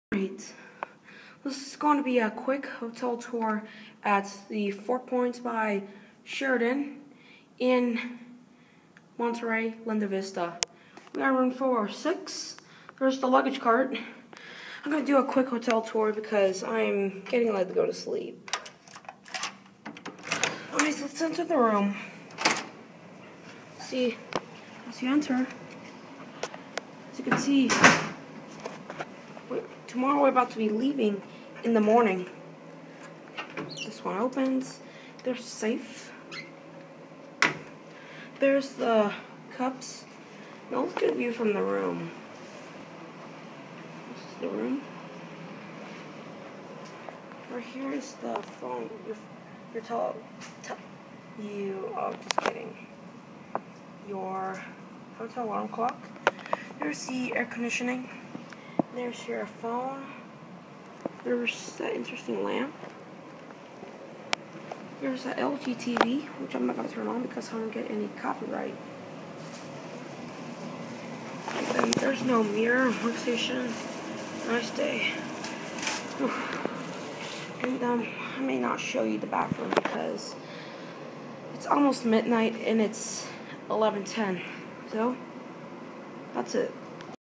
Hotel Room Tour: Four Points By Sheraton in Monterrey Lindavista.
I was Tired So I Had To Do A Quick Hotel Room Tour.